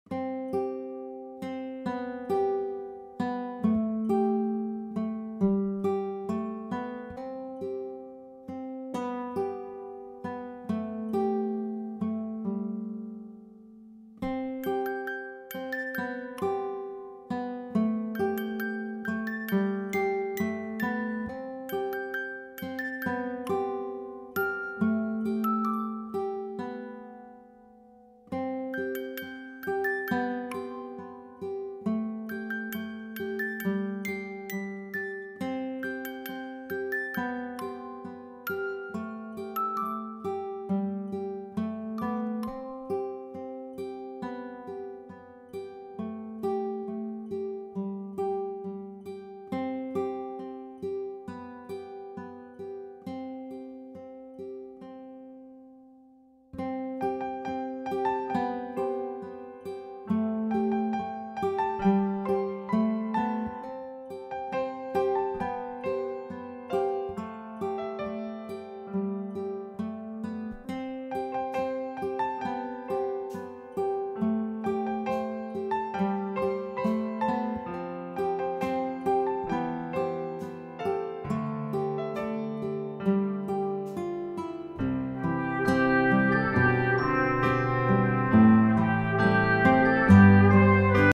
Find Text Number in excel | Excel for auditing & auditors
This excel tutorial video shows you how to find text number using simple trick in ms excel.